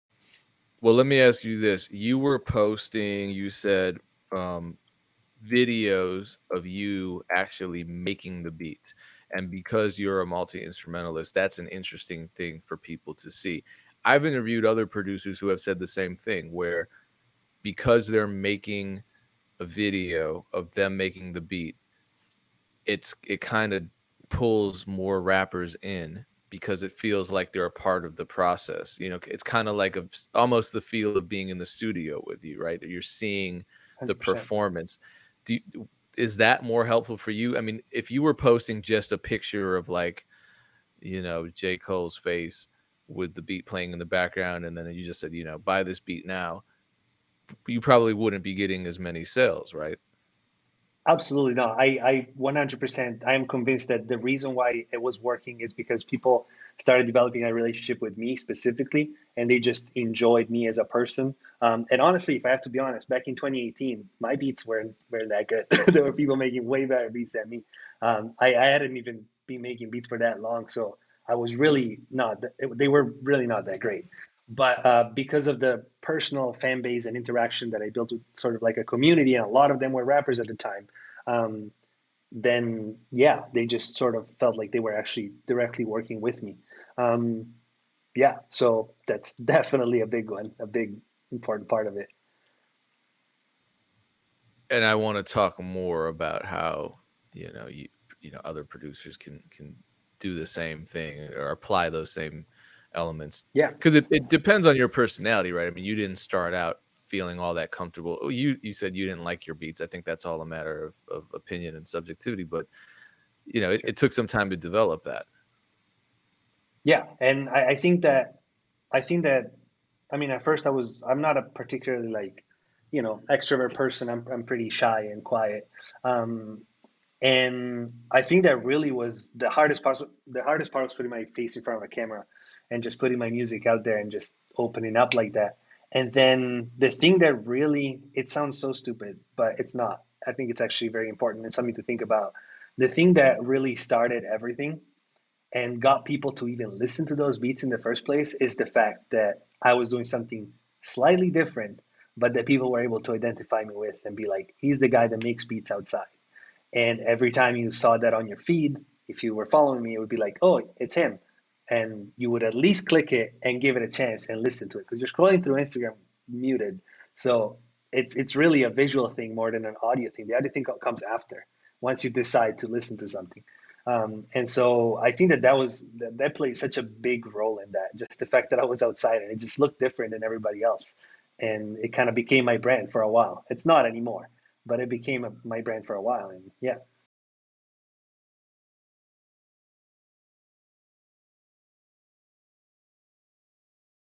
How Producers Build an Online Community | Ed Talenti
Ed Talenti explains the importance of building an online community on social media.